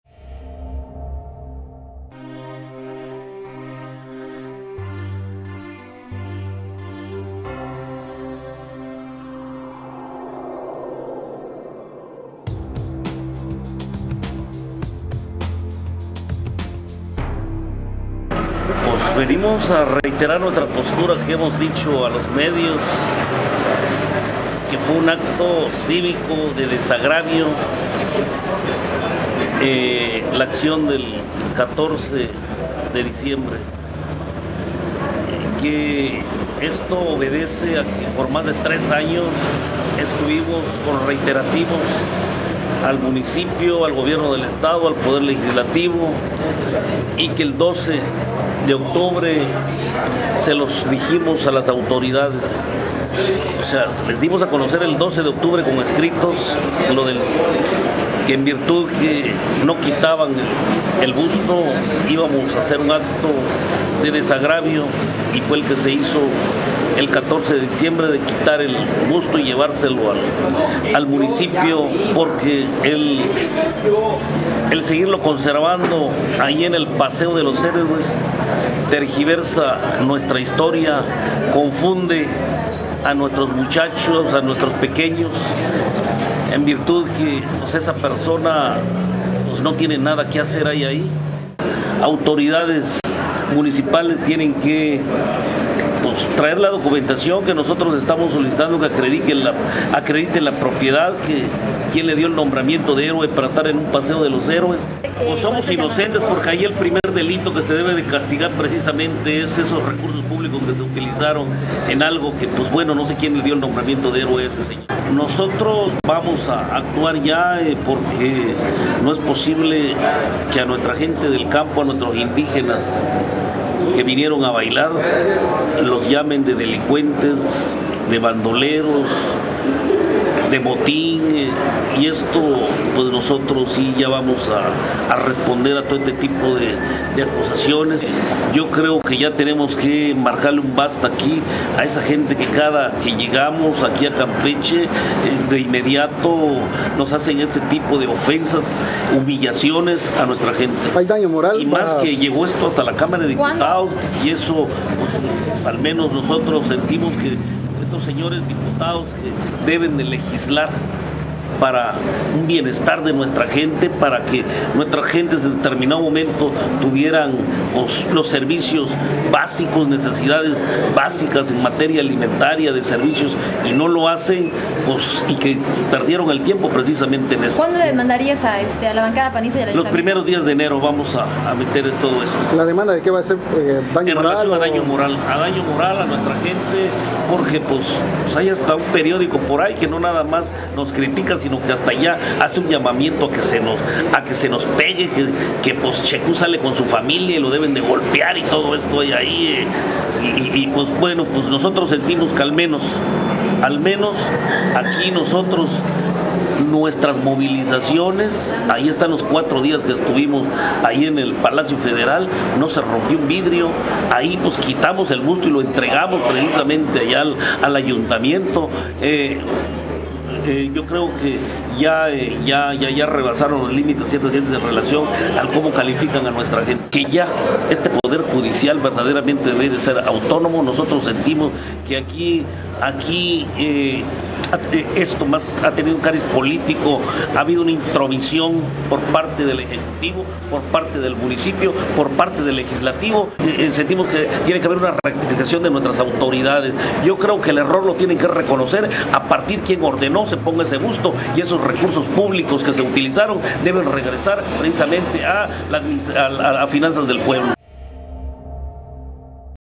Luis Antonio Che Cu contrademandará por daño moral
Tras reafirmar que el haber tirado de su sitio el busto de Juan Camilo Mouriño fue un acto de desagravio cultural, el líder del FRECIEZ, Luis Antonio Ce Cu, informó que contrademandará por "daño moral" a políticos y todos aquellos que los han llamado "pandilleros", incluidos algunos periodistas que incitan a la ciudadanía a un  linchamiento público.